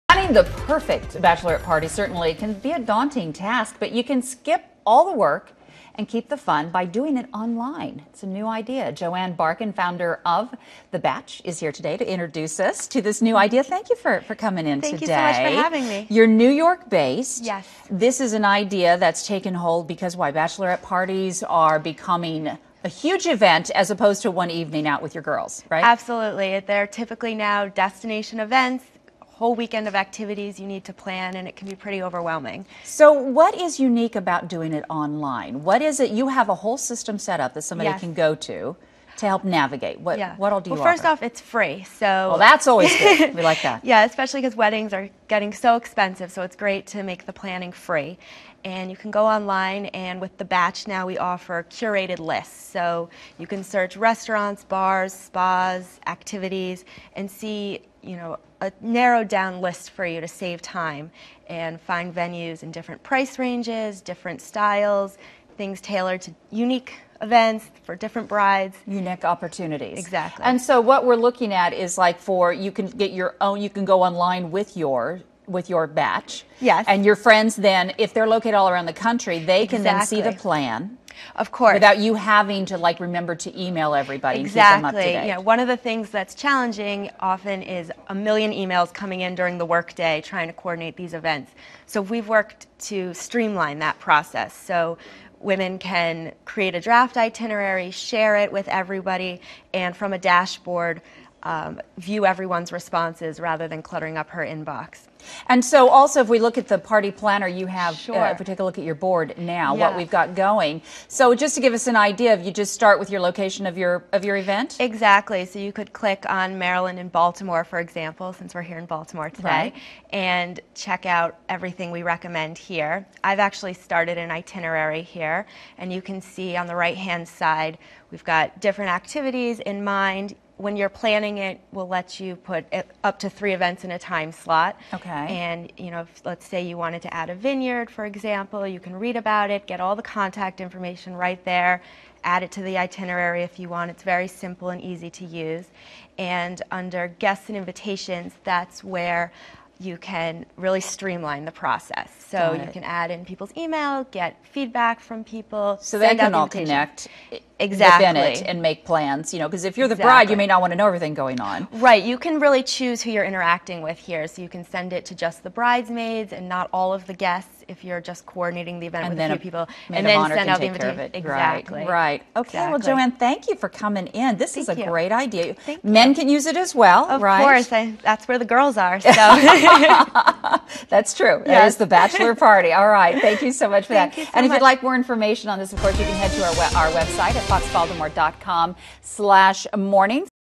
Bachelorette Party Planning
Planning the perfect bachelorette party can sometimes be a daunting task. But you can skip the work and keep the fun by planning online. Joanne Barken, founder of "The Back" introduces an online platform that simplifies the process.